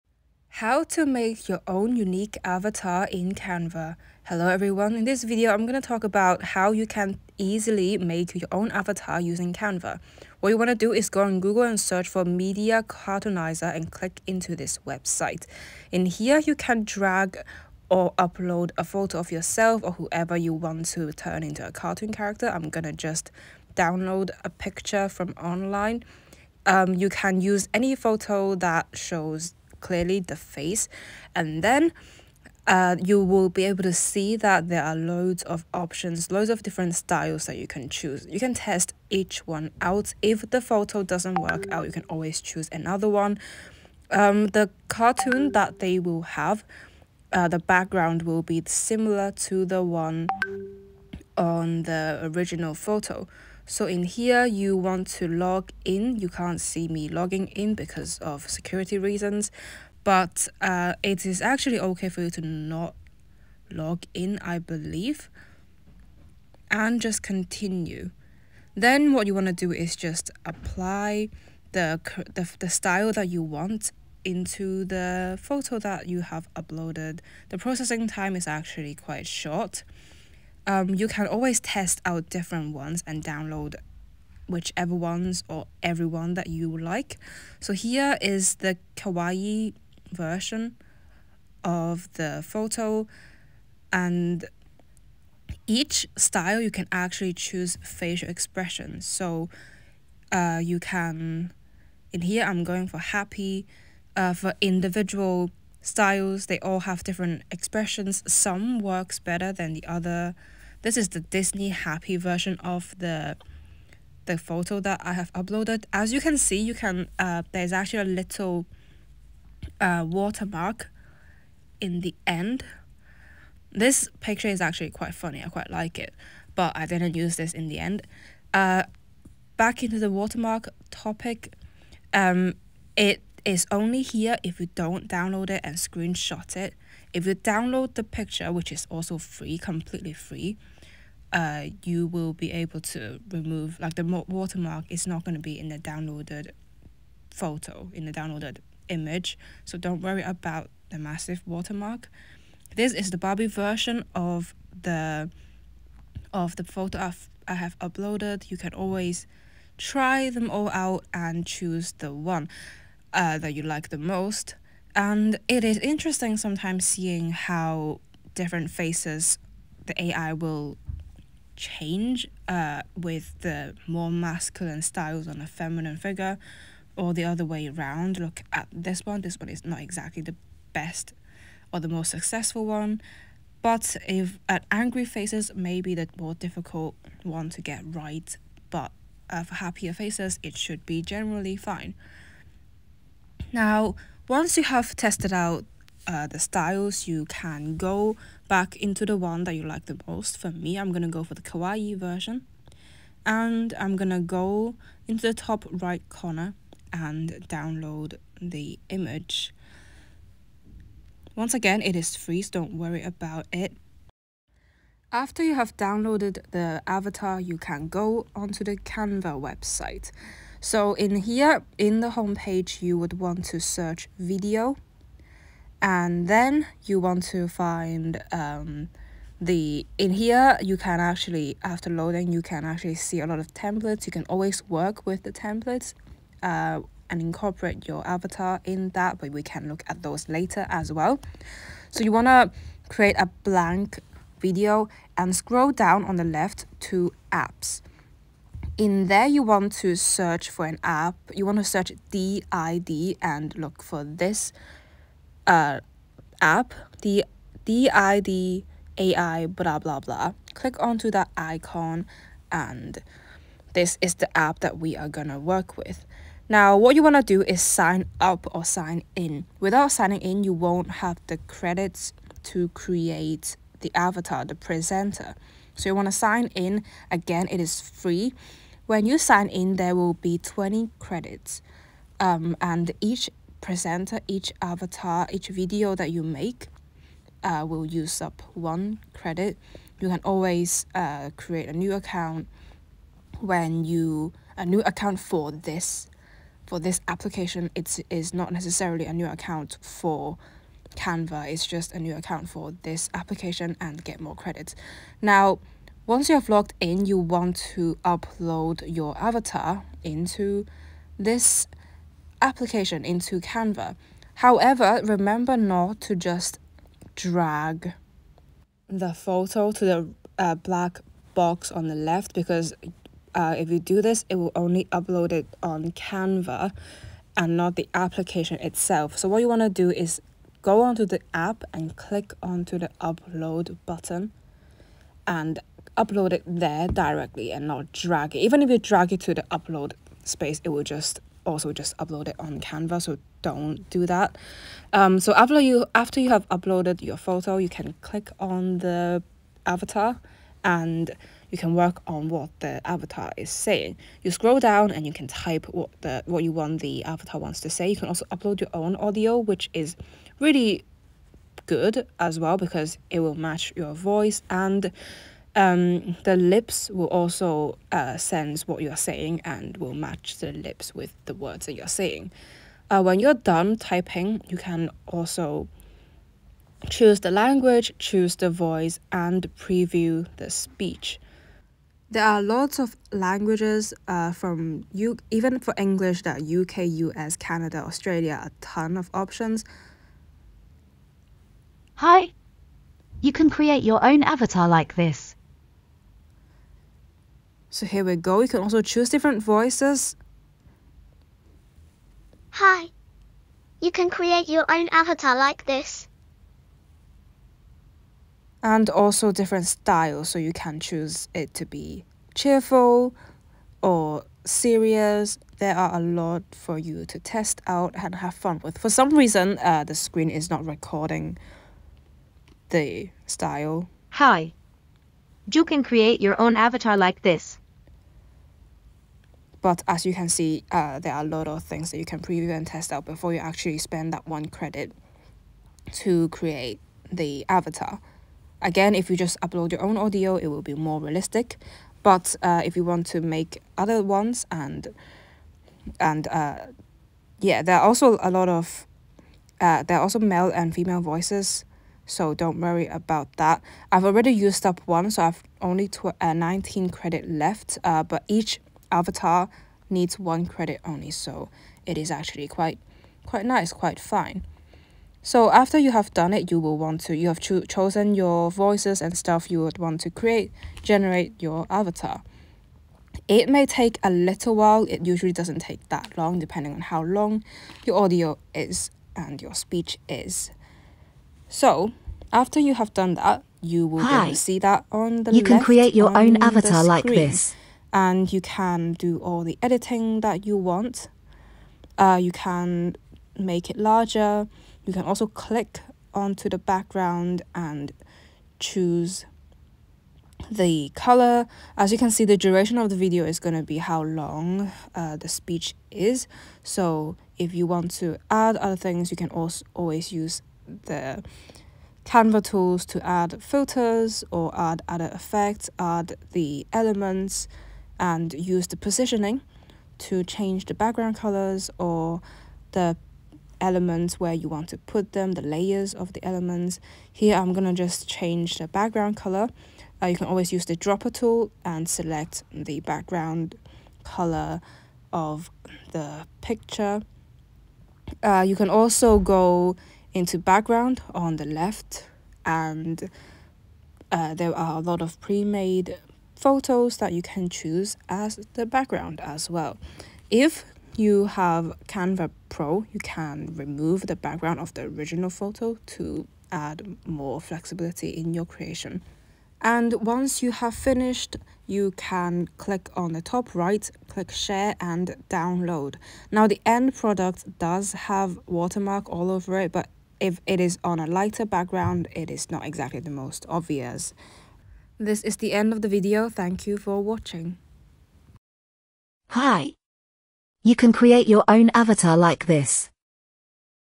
How to Create Your Unique Avatar in Canva | Make AI Avatar in Canva (Tutorial 2025)
If you are looking for a video about how to create your unique avatar in canva, here it is!
In this video you'll learn how to make ai avatar in canva.